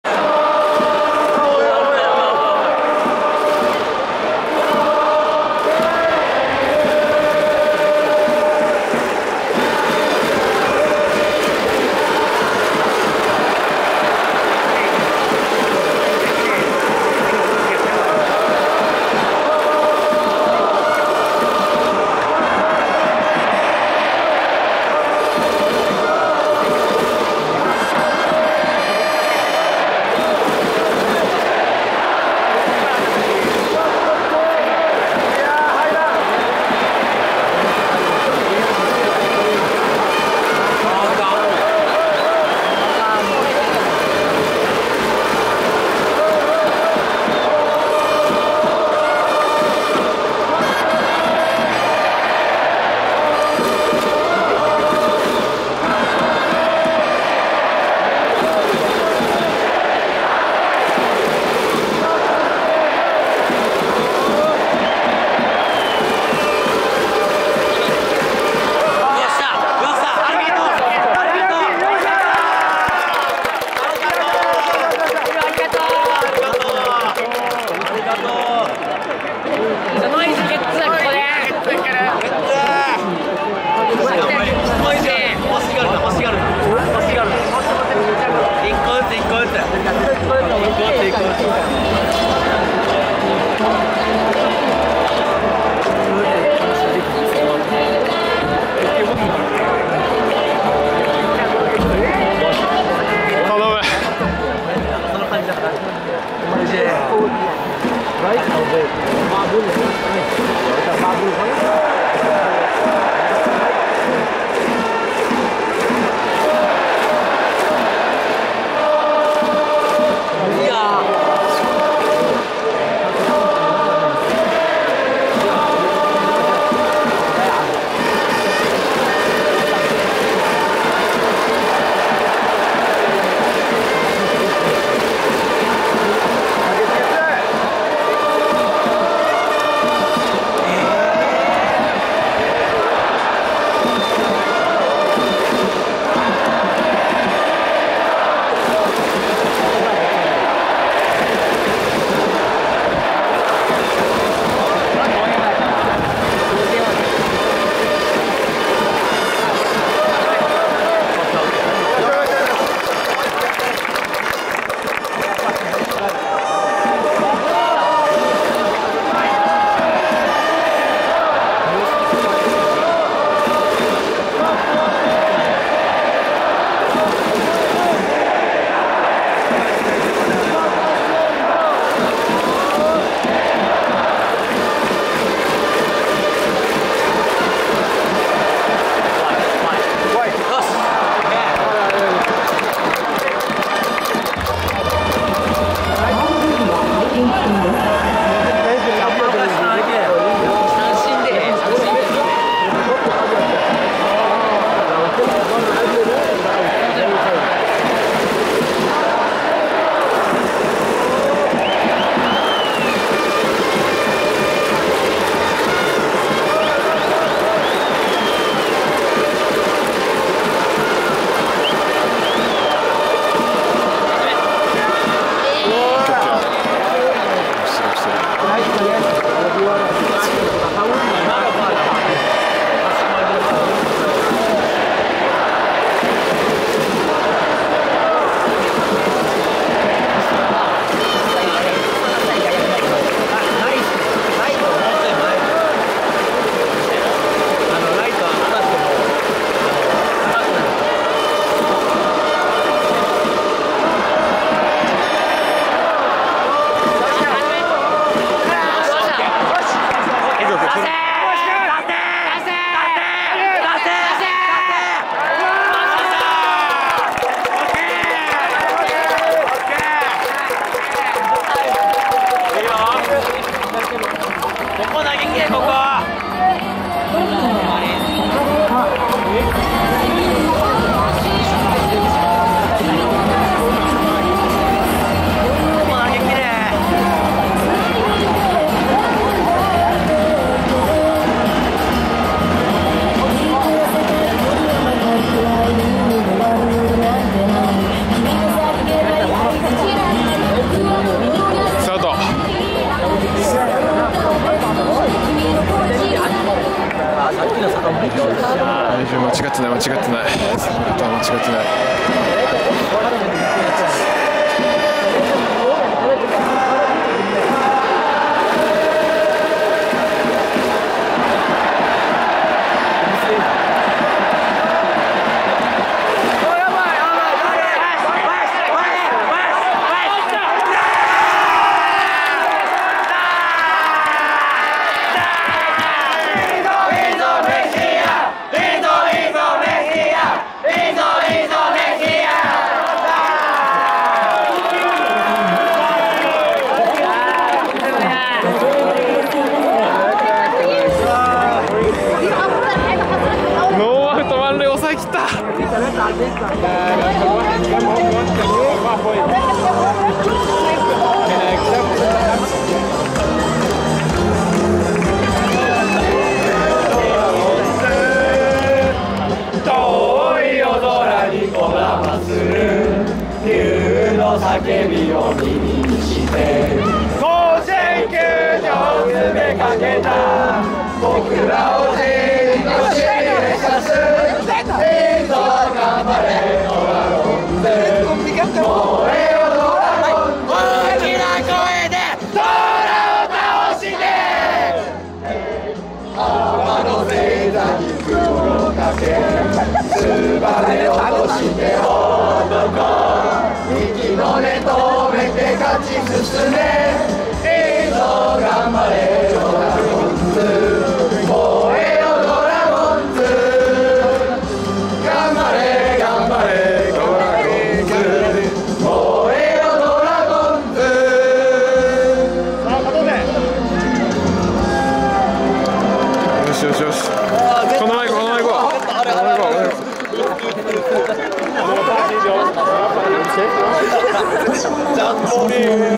2023プロ野球 無死満塁の大ピンチを抑え、雄叫びを上げるメヒア
佐藤　三飛（1:03）
ノイジー　中飛（4:11）
坂本　中飛（5:18）